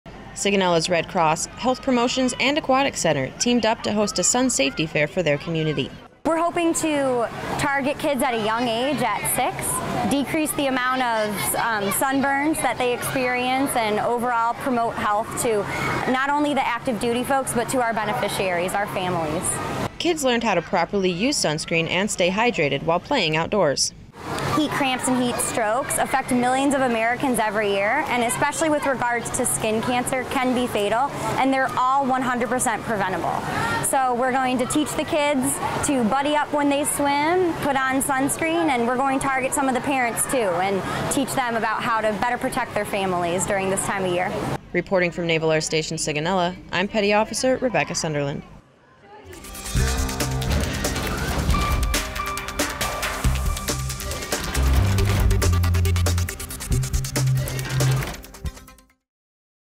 Sun Safety
Sigonella's youth learn about sun safety.